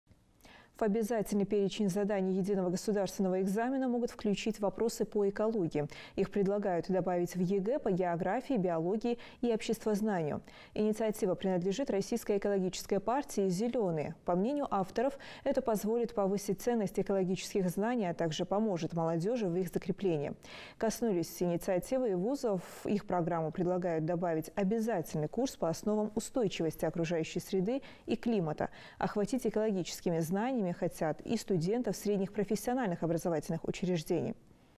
В России предложили включить в ЕГЭ вопросы по экологии
В обязательный перечень заданий Единого государственного экзамена могут включить вопросы по экологии.

Их предлагают добавить в ЕГЭ по географии, биологии и обществознанию. Инициатива принадлежит российской экологической партии «Зеленые».

По мнению авторов, это позволит повысить ценность экологических знаний, а также поможет молодежи в их закреплении.

Коснулась инициатива и вузов - в их программу предлагают добавить обязательный курс по основам устойчивости окружающей среды и климата. Охватить экологическими знаниями хотят и студентов средних профессиональных образовательных учреждений.